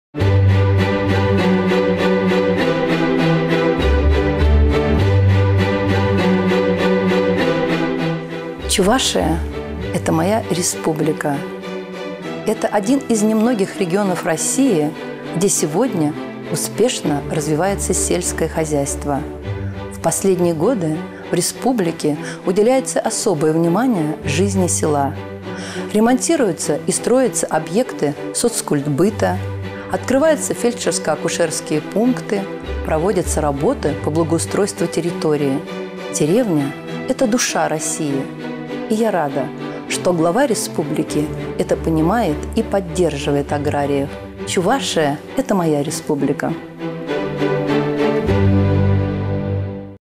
Ирина Письменская
Директор Чувашского регионального филиала ОАО "Россельхозбанк"